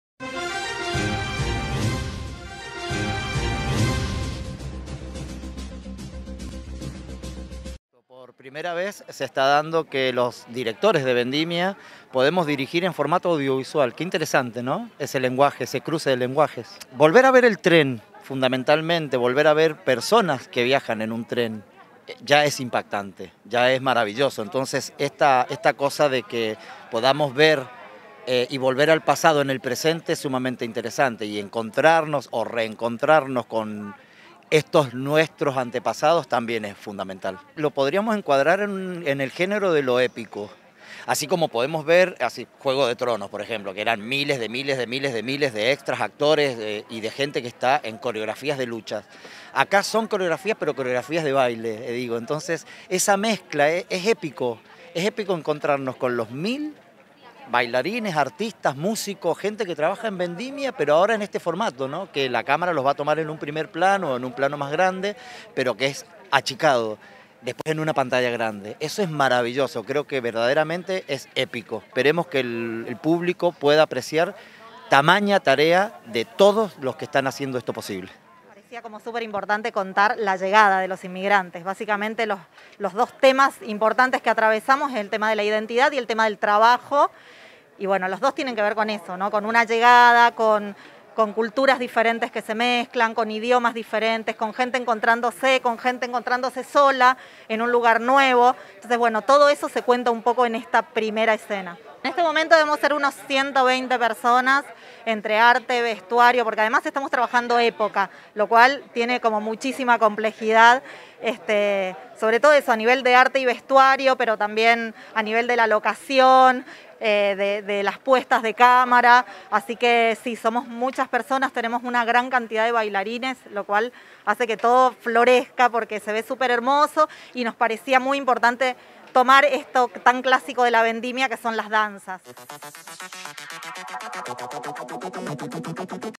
EDICIÓN ESPECIAL
Alejandro Conte, director artístico dijo estar preparado para dirigir en formatos digitales y nos cuenta la emoción de volver a ver el tren y a sus pasajeros.